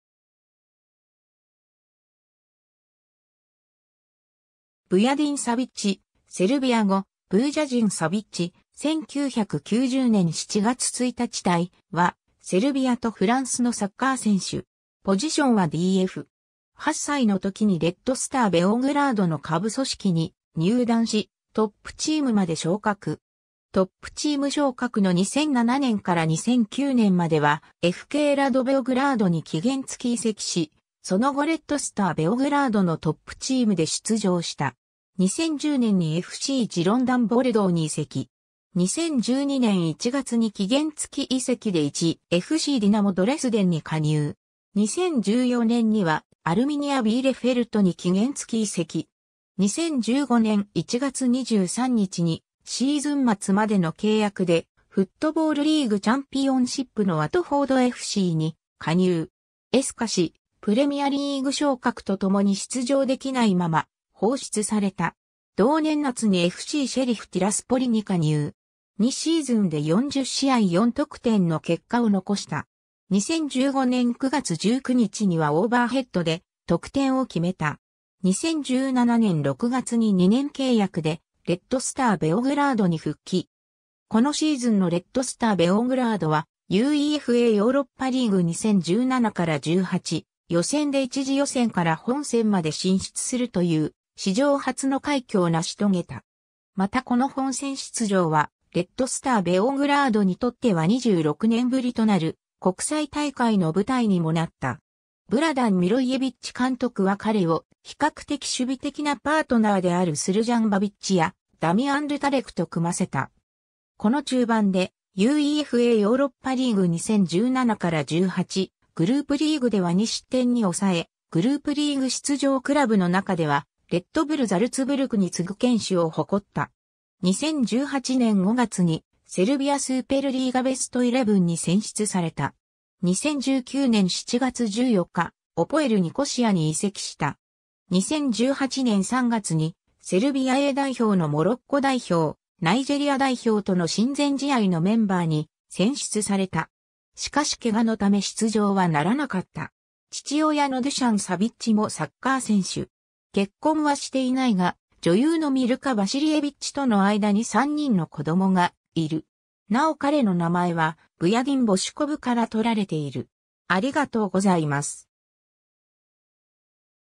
ヴヤディン・サヴィッチ
レッドスター・ベオグラードの選手
FKラド・ベオグラードの選手
FCジロンダン・ボルドーの選手
ディナモ・ドレスデンの選手
アルミニア・ビーレフェルトの選手
FCシェリフ・ティラスポリの選手
ヴヤディン・サヴィッチ (セルビア語: Вујадин Савић, Vujadin Savić、1990年7月1日 - ）は、セルビアとフランスのサッカー選手。
 8歳の時にレッドスター・ベオグラードの下部組織に入団し、トップチームまで昇格。
トップチーム昇格の2007年から2009年まではFKラド・ベオグラードに期限付き移籍し、その後レッドスター・ベオグラードのトップチームで出場した。
 2010年にFCジロンダン・ボルドーに移籍。
2012年1月に期限付き移籍で1.FCディナモ・ドレスデンに加入。
2014年にはアルミニア・ビーレフェルトに期限付き移籍。
 2015年1月23日にシーズン末までの契約でフットボールリーグ・チャンピオンシップのワトフォードFCに加入。
ｓかしプレミアリーグ昇格とともに出場出来ないまま放出された。
 同年夏にFCシェリフ・ティラスポリに加入。
2シーズンで40試合4得点の結果を残した。
2015年9月19日にはオーバーヘッドで得点を決めた。
 2017年6月に2年契約でレッドスター・ベオグラードに復帰。
このシーズンのレッドスター・ベオグラードはUEFAヨーロッパリーグ 2017-18 予選で1次予選から本戦まで進出するという史上初の快挙を成し遂げた。
またこの本戦出場はレッドスター・ベオグラードにとっては26年ぶりとなる国際大会の舞台にもなった。
ヴラダン・ミロイェヴィッチ監督は彼を比較的守備的なパートナーであるスルジャン・バビッチやダミアン・ル・タレクと組ませた。
この中盤でUEFAヨーロッパリーグ 2017-18 グループリーグでは2失点に抑え、グループリーグ出場クラブの中ではレッドブル・ザルツブルクに次ぐ堅守を誇った。
2018年5月にセルビア・スーペルリーガベストイレブンに選出された。
 2019年7月14日、APOELニコシアに移籍した。
 2018年3月にセルビアA代表のモロッコ代表、ナイジェリア代表との親善試合のメンバーに選出された。
しかし怪我のため出場はならなかった。
 父親のドゥシャン・サヴィッチもサッカー選手。
結婚はしていないが、女優のミルカ・ヴァシリェヴィッチとの間に3人の子供がいる。
なお彼の名前はヴヤディン・ボシュコヴから取られている。